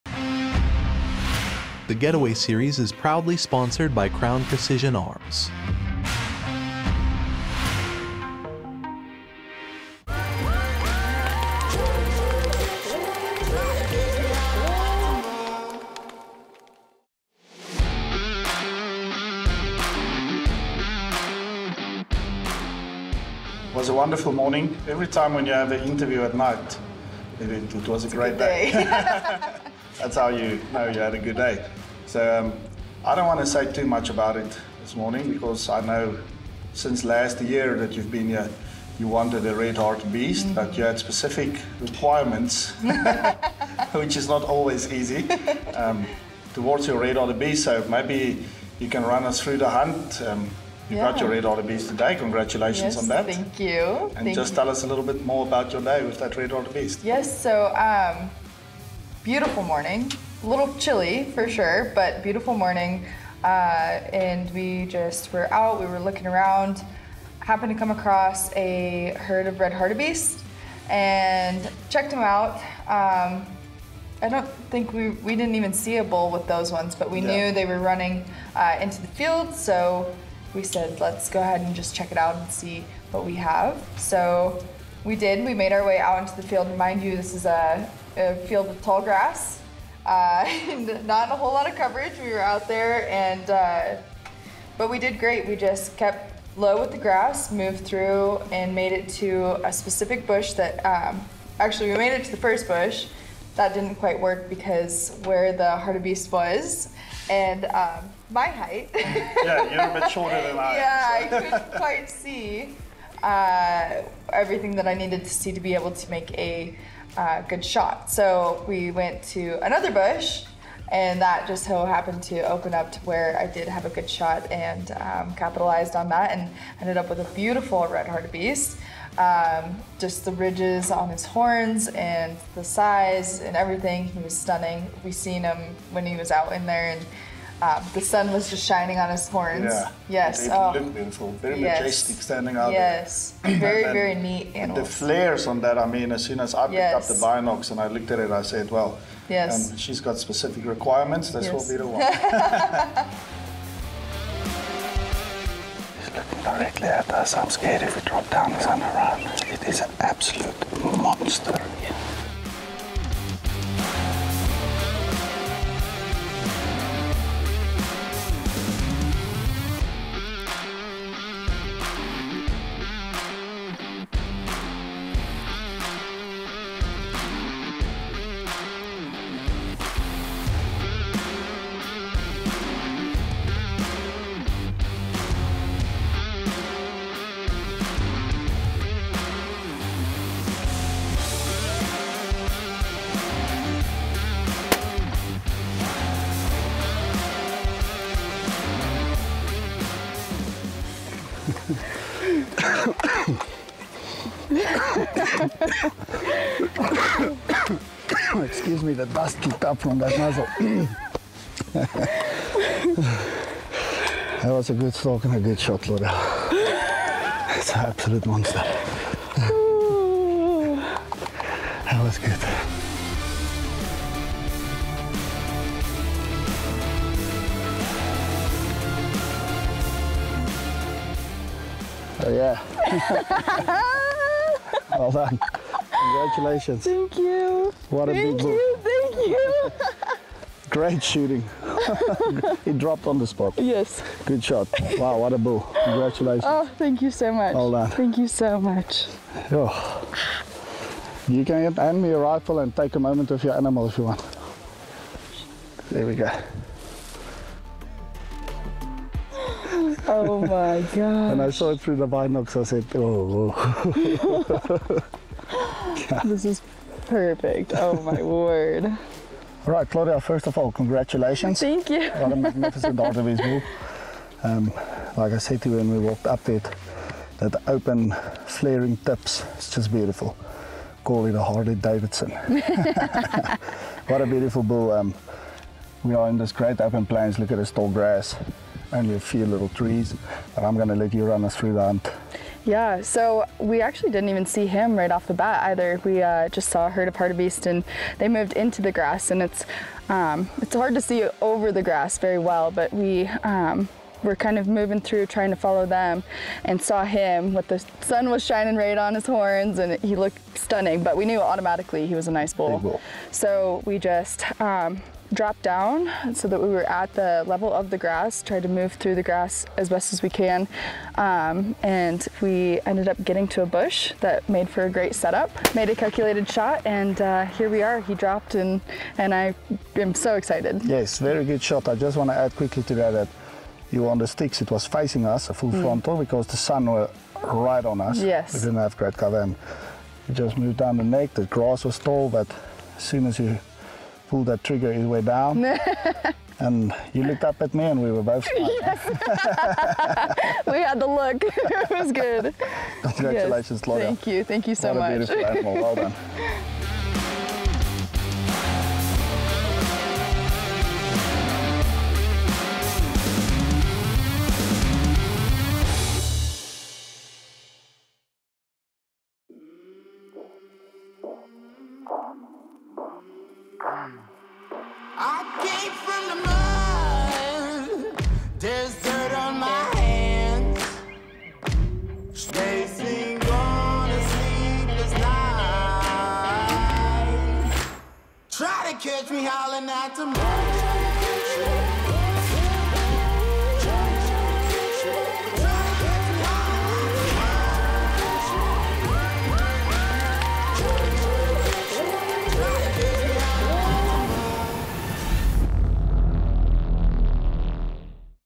Hunting the Red Hartebeest in Namibia
The red hartebeest (Alcelaphus buselaphus caama) is a large African antelope species known for its striking reddish-brown coat and distinctively shaped horns. Hunting a Red Hartebeest is a very rewarding hunt and definitely a must shoot animal when you come and hunt in africa.